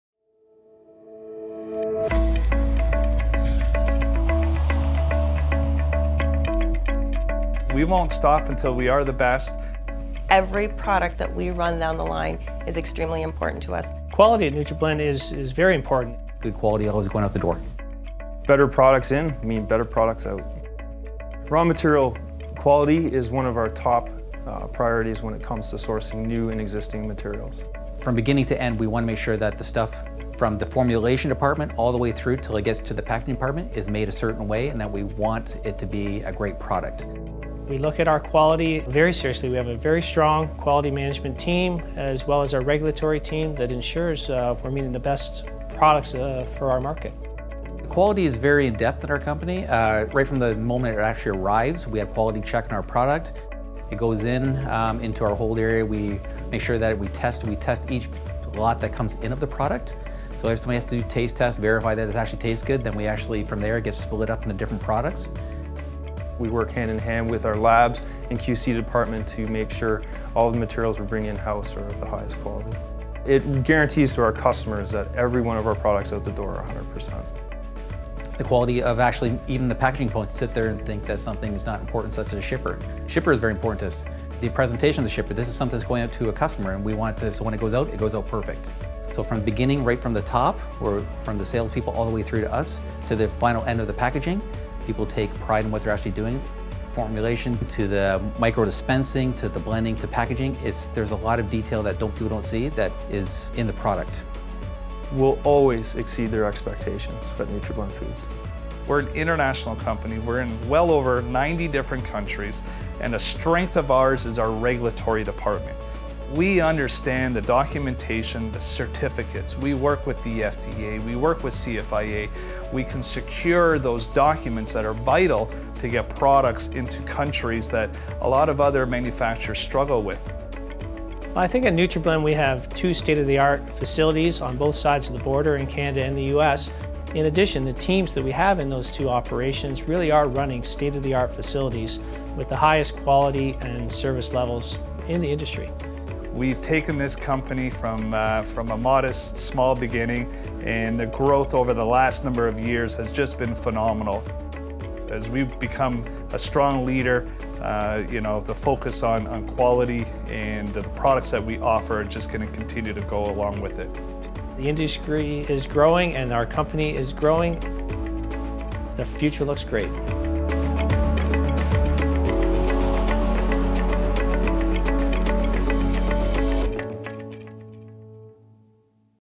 NutraBlend Foods: Stringent quality controls in place.
At NutraBlend Foods, we ensure stringent quality controls for consistency and compliance regardless of what we are manufacturing. We have the highest certifications needed for the manufacturing of protein powders in our industry.